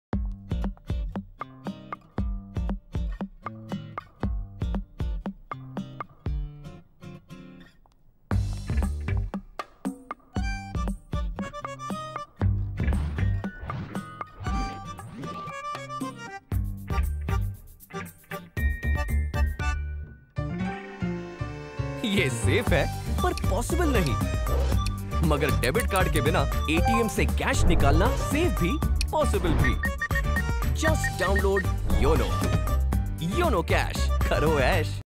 Experience a secure ATM transaction with YONO Cash
Embrace the digital lifestyle with YONO. Facilitate secure cardless cash withdrawal with YONO Cash.